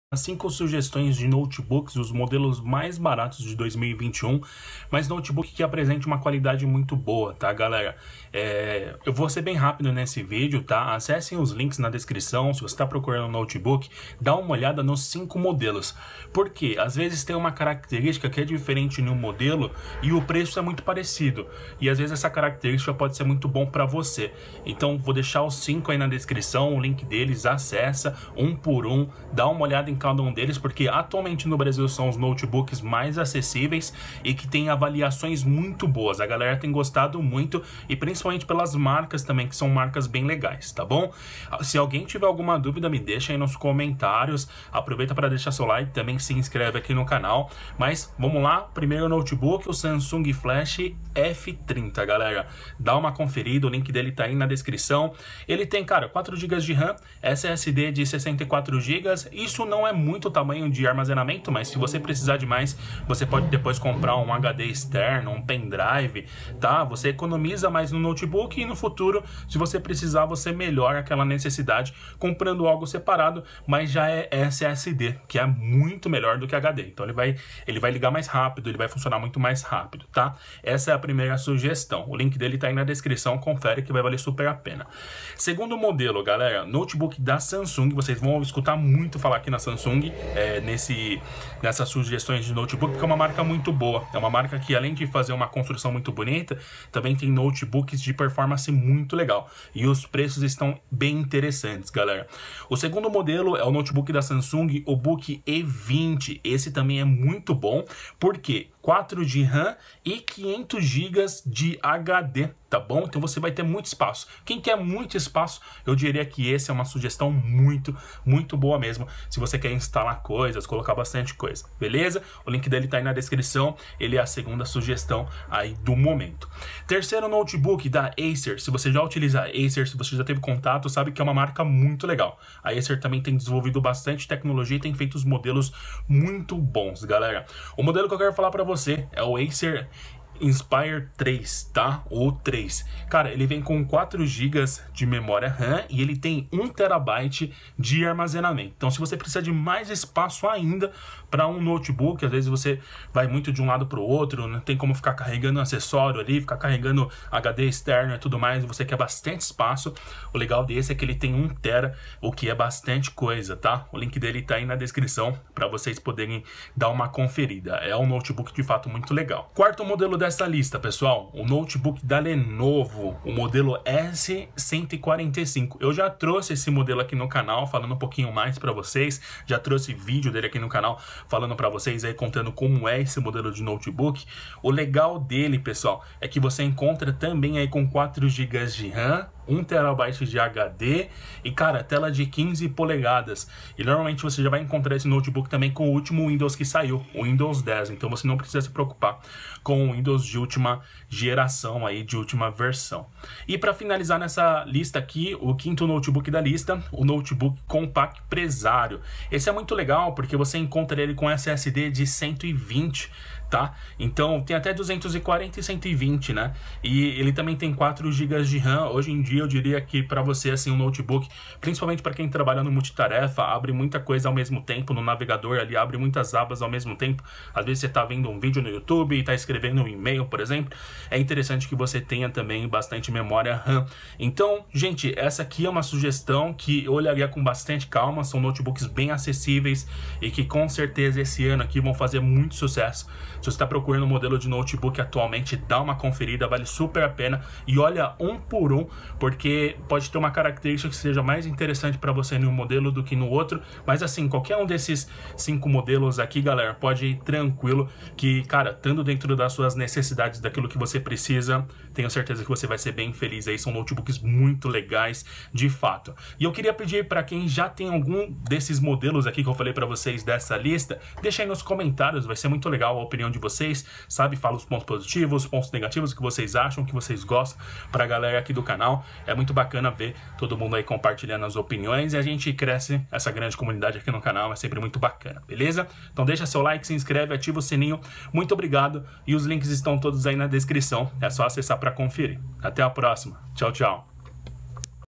Os NOTEBOOK 💻 Mais Baratos de 2021 BRASIL TOP 5
Produto: 1 Notebook Samsung Flash F30
2 Notebook Samsung Book E20
3 Acer Aspire 3 A315-34-C6ZS
4 Notebook Lenovo IdeaPad S145
5 Notebook Compaq Presario